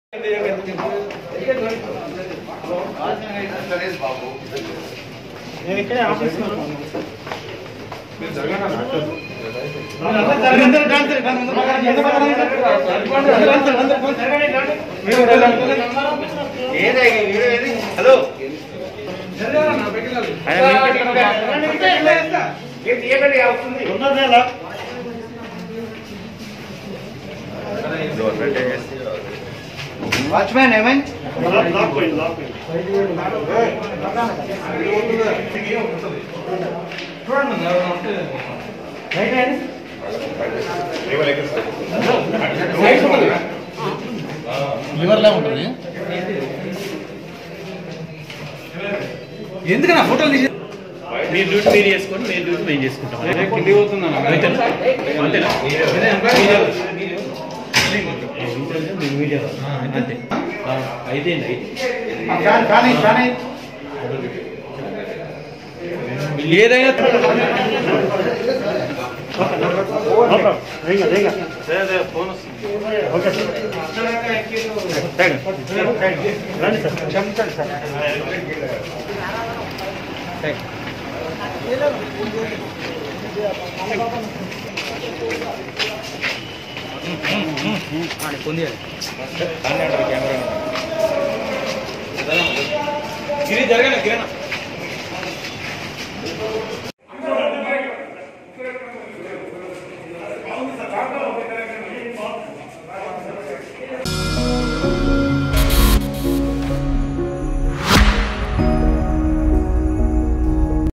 minister koppula eashwar stuck in lift at hyderabad || Samayam Telugu
లిఫ్ట్ లో ఇరుక్కుపోయిన మంత్రి కొప్పుల ఈశ్వర్
minister koppula eashwar stuck in lift at hyderabad Rescued after 30 minutes